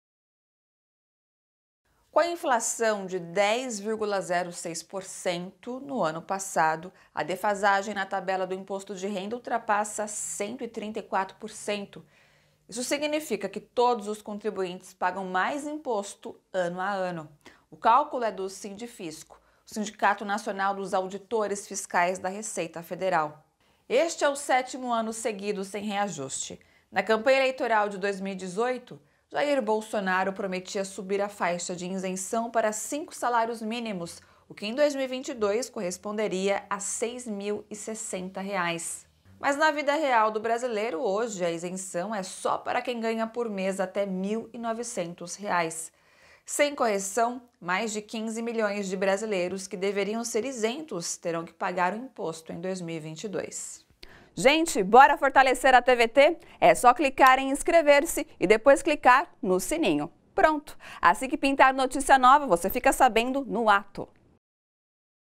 Tabela defasada faz brasileiros pagarem cada vez mais Imposto de Renda
Com a inflação de 10,06%, no ano passado, a defasagem na tabela do Imposto de Renda ultrapassa 134%. Isso  significa que todos os contribuintes pagam mais imposto ano a ano.